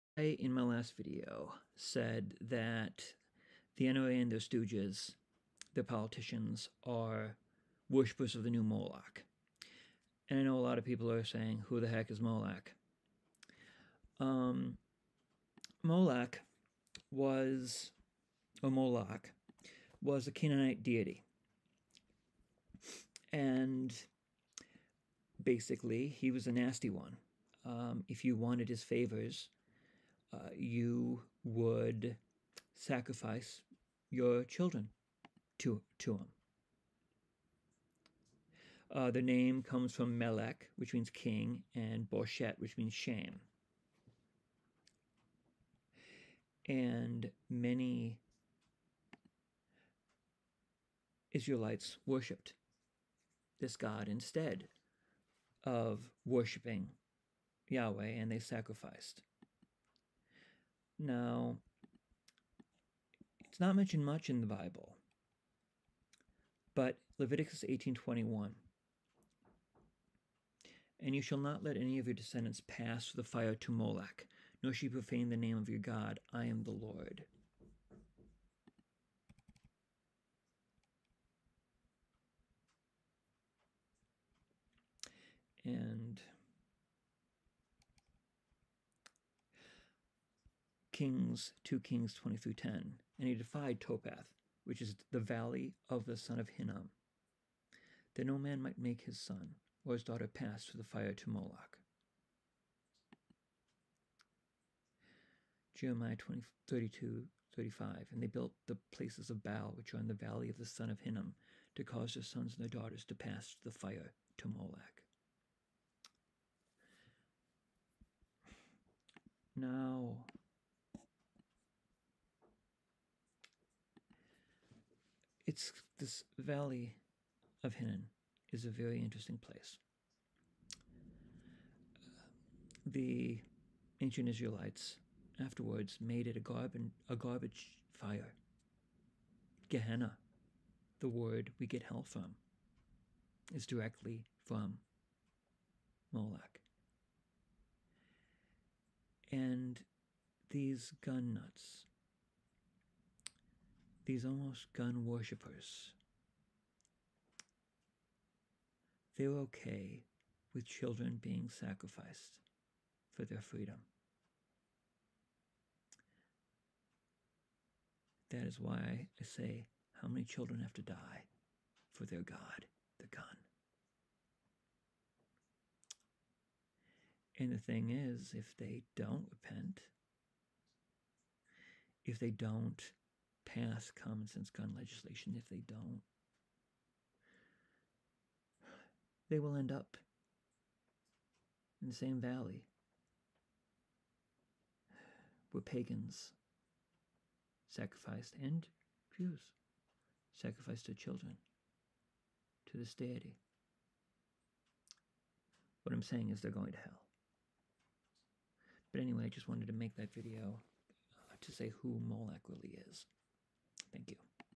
Molech, The NRA, their stooges and Hell
In my last video I said that gun and gun rights are like the modern Molech. This video is to explain who Molech was why the Gun or maybe even the twisted idea of "freedom" they worship is the modern day Molech